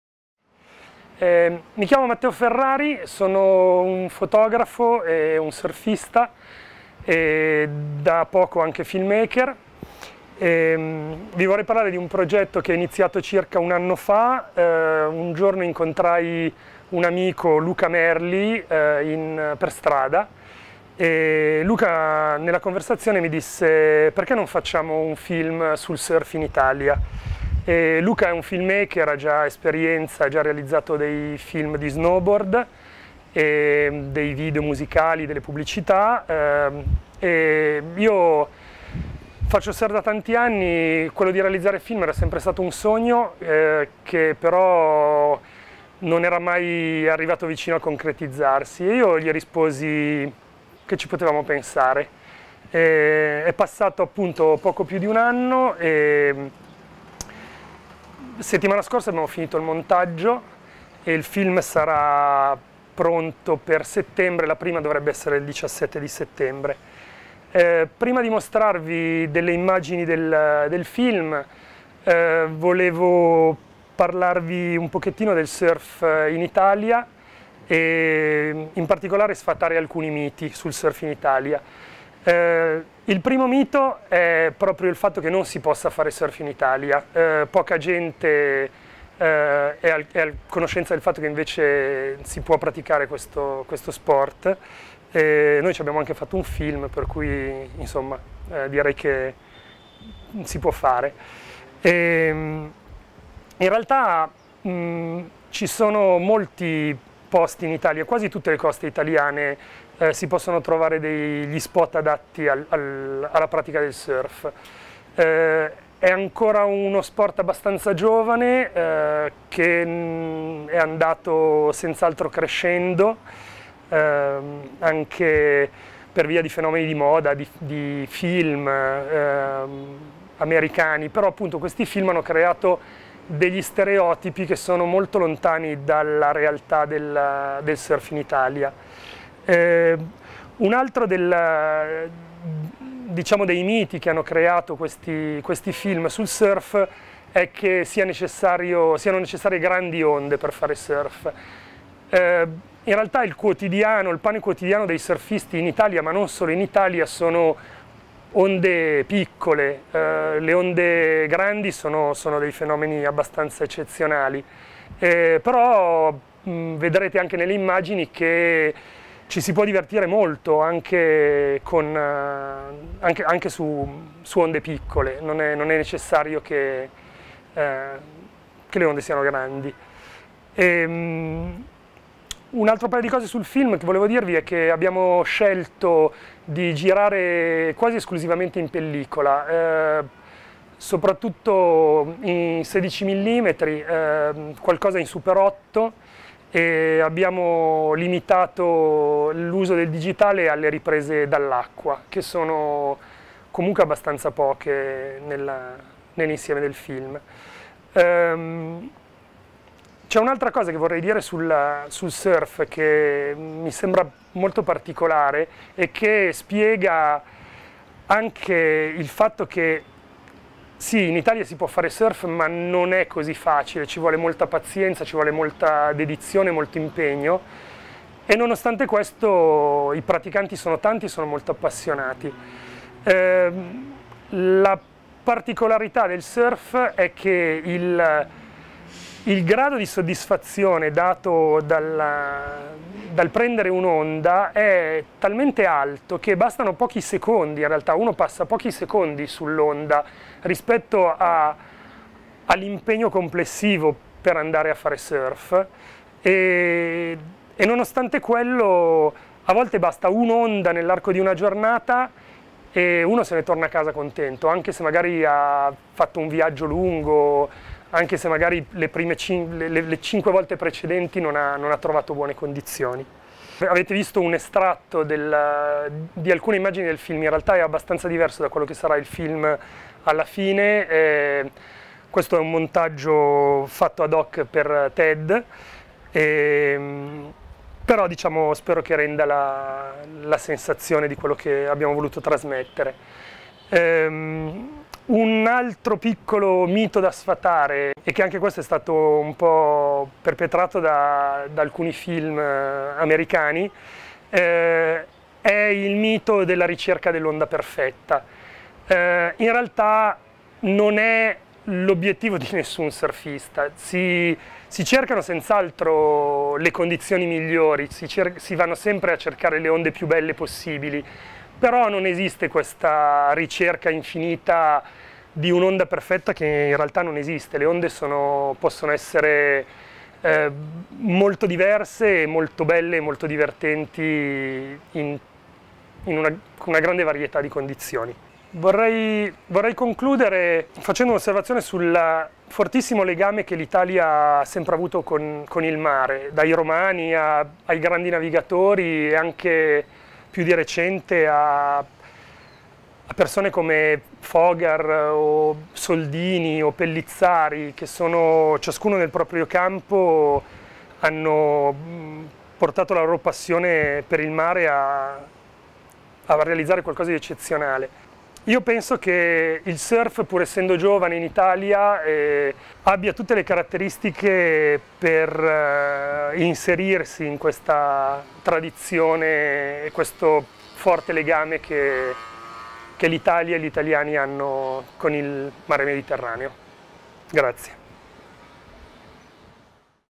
TEDxPalermo - Matteo Ferrari - Surfing Italian Waves
Matteo is a photographer and a surfer. In 2010, together with a  friend and colleague Luca Merli, his two passions eventually match in the making of the first documentary about surfing in Italy, "Onde Nostre" ( traduced sounds like: "Our own Waves") entirely shot in 16mm film. Aside from his assignments in fashion and reportage photography, he works on his own several projects, among which Automotive Monogamy, winner of the 2007 ARTE Prize.
This independent TEDx event is operated under license from TED.
This event is not being organized by the TED conferences -- this is an independently organized TED event.

Thank You to Margherita Bruno for translation.